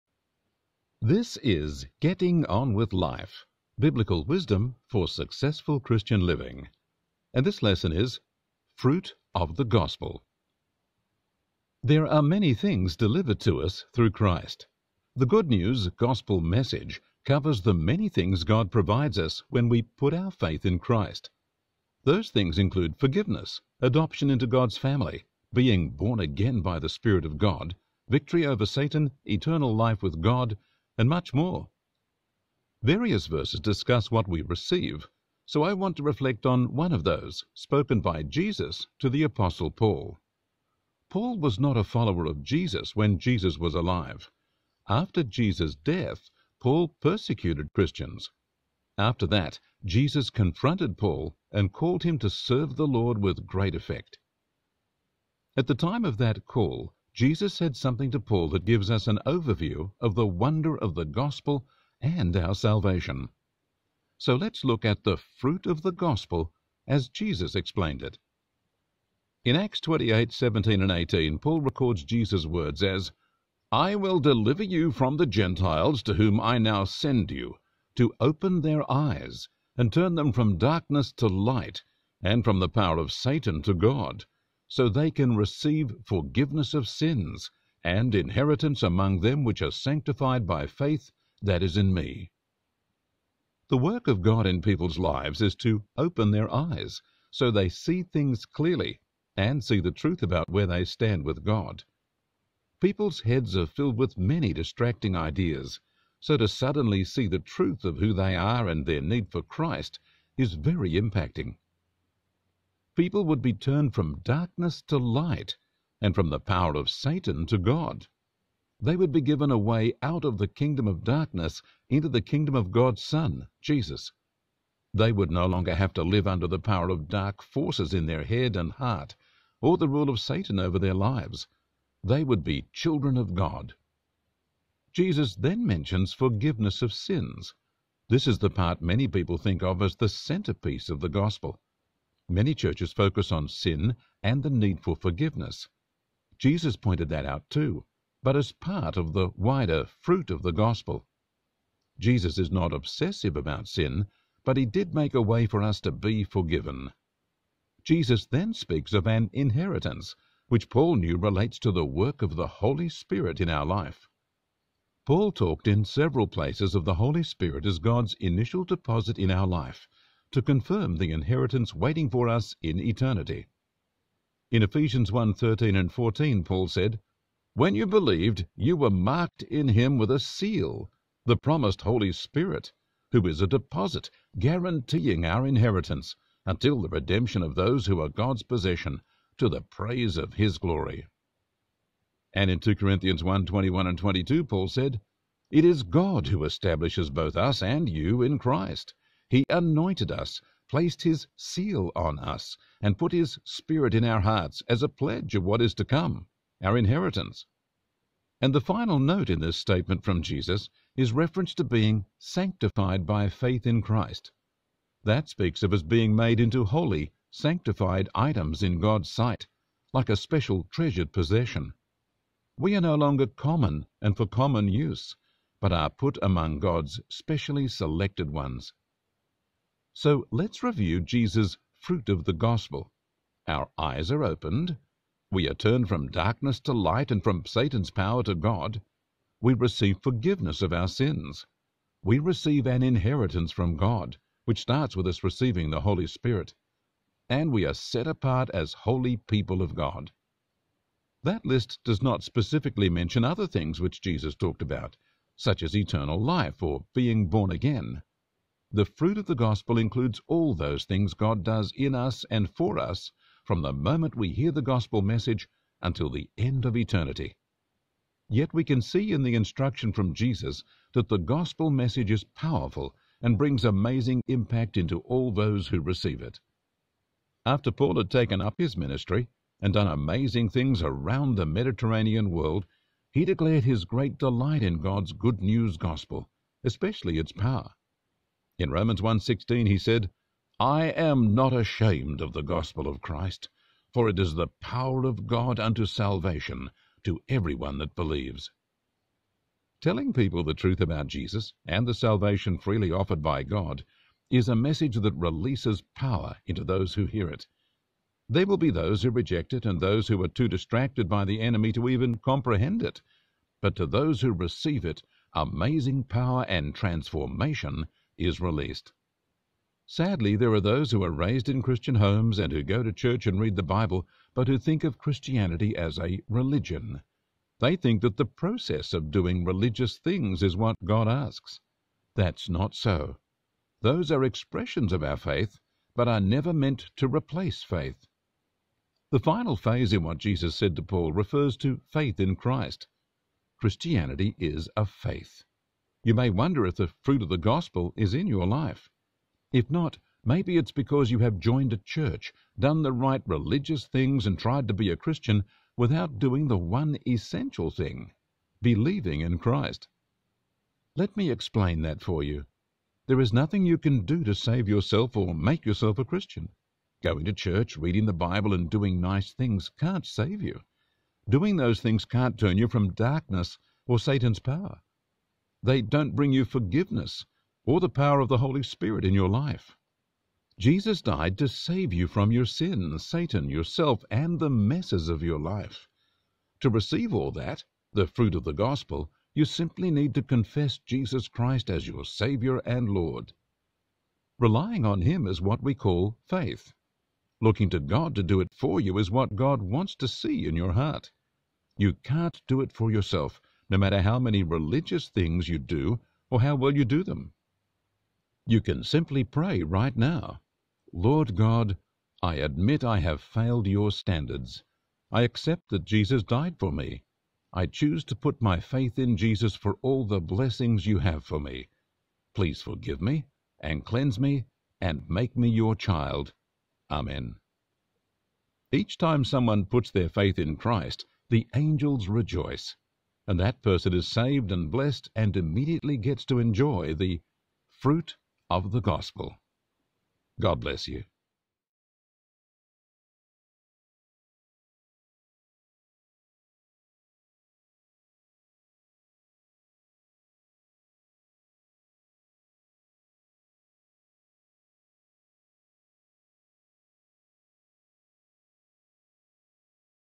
Fruit of the Gospel. Ideal for Bible Study, Church and Personal Devotions.
Note the summary of the Gospel given by Jesus to Paul, who did not follow Jesus when Jesus was alive. After Jesus’ death Paul persecuted Christians. Then Jesus called Paul to serve the Lord.
In that call Jesus gave us an overview of our salvation.
“I will deliver you from the Gentiles to whom I now send you, to open their eyes and turn them from darkness to light, and from the power of Satan to God, so they can receive forgiveness of sins, and inheritance among them which are sanctified by faith that is in me.” Acts 28:17,18
God opens people's eyes to see where they stand with God. People have many distracting ideas, so suddenly seeing truth of their need for Christ is very impacting.
People would be turned from darkness to light and from Satan's power to God. They would be freed from the Kingdom of Darkness into the Kingdom of God’s Son. They would no longer live under the rule of Satan. They would be God's children.
Jesus then mentions forgiveness. He made a way for us to be forgiven.
Jesus then speaks of inheritance, which relates to the work of the Spirit. Paul called the Spirit God’s initial deposit in our life, to confirm the inheritance waiting for us in eternity.
“When you believed, you were marked in him with a seal, the promised Holy Spirit, who is a deposit guaranteeing our inheritance until the redemption of those who are God’s possession—to the praise of his glory.” Eph 1:13,14
“It is God who establishes both us and you in Christ. He anointed us, placed His seal on us, and put His Spirit in our hearts as a pledge of what is to come (our inheritance).” 2Cor 1:21,22
And the final note in this statement from Jesus is reference to being ‘sanctified by faith’ in Christ. That speaks of us being made into holy (sanctified) items in God’s sight, like a special treasured possession. We are no longer common and for common use, but are put among God’s specially selected ones.
So let’s review Jesus’ list of the Fruit of the Gospel: Our eyes are opened; We are turned from darkness to light and from Satan’s power to God; We receive forgiveness of our sins; We receive an inheritance from God, which starts with us receiving the Holy Spirit; and we are set apart as holy people of God.
That list does not specifically mention other things which Jesus talked about, such as eternal life or being born again. The Fruit of the Gospel includes all those things God does in us and for us from the moment we hear the Gospel message until the end of eternity. Yet we can see in the instruction from Jesus that the Gospel message is powerful and brings amazing impact into all those who receive it.
After Paul had taken up his ministry and done amazing things around the Mediterranean world he declared his great delight in God’s good news Gospel, especially its power.
“I am not ashamed of the gospel of Christ: for it is the power of God unto salvation to every one that believes” Rom 1:16
Telling people the truth about Jesus and the salvation freely offered by God is a message that releases power into those who hear it. There will be those who reject it and those who are too distracted by the enemy to even comprehend it, but to those who receive it amazing power and transformation is released.
Sadly, there are those who are raised in Christian homes and who go to church and read the Bible, but who think of Christianity as a religion. They think that the process of doing religious things is what God asks. That’s not so. Those are expressions of our faith, but are never meant to replace faith.
The final phrase in what Jesus said to Paul refers to faith in Christ. Christianity is a faith.
You may wonder if the Fruit of the Gospel is in your life. If not, maybe it’s because you have joined a church, done the right religious things and tried to be a Christian, without doing the one essential thing, believing in Christ.
Let me explain that for you. There is nothing you can do to save yourself or make yourself a Christian. Going to church, reading the Bible and doing nice things can’t save you. Doing those things can’t turn you from darkness or Satan’s power. They don’t bring you forgiveness or the power of the Holy Spirit in your life.
Jesus died to save you from your sin, Satan, yourself, and the messes of your life. To receive all that, the Fruit of the Gospel, you simply need to confess Jesus Christ as your saviour and Lord. Relying on Him is what we call faith. Looking to God to do it for you is what God wants to see in your heart. You can’t do it for yourself, no matter how many religious things you do or how well you do them.
You can simply pray right now, “Lord God, I admit I have failed your standards. I accept that Jesus died for me. I choose to put my faith in Jesus for all the blessings you have for me. Please forgive me, and cleanse me and make me Your child. Amen.”
Each time someone puts their faith in Christ the angels rejoice. And that person is saved and blessed and immediately gets to enjoy the Fruit of the Gospel.